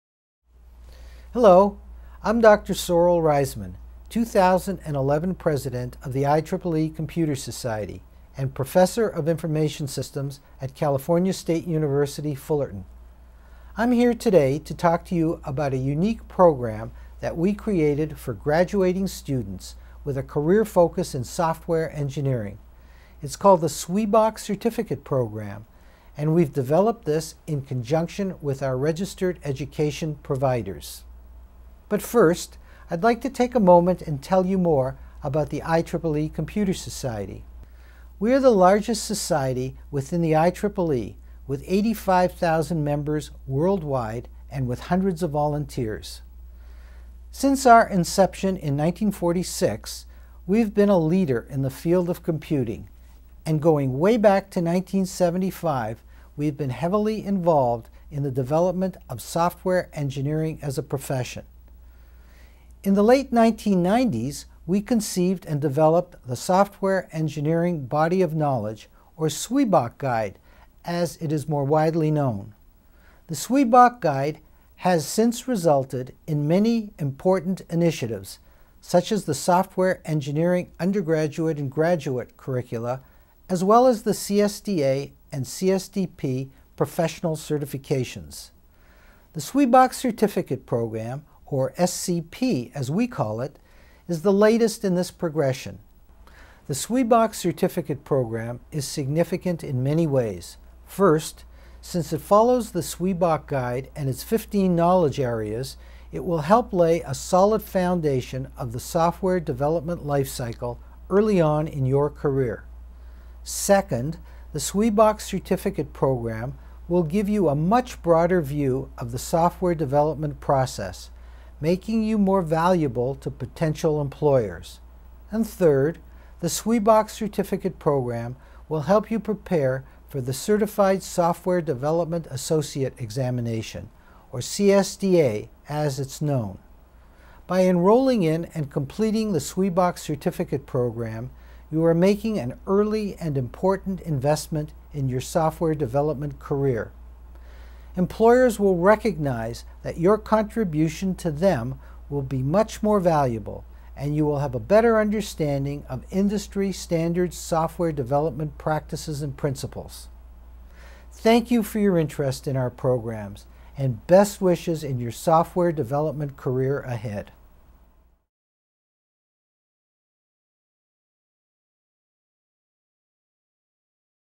IEEE Computer Society SWEBOK Certificate Program (SCP) benefits for students
Sorel Reisman, 2011 IEEE Computer Society President, discusses the SWEBOK Certificate Program (SCP) and the benefit for software engineering students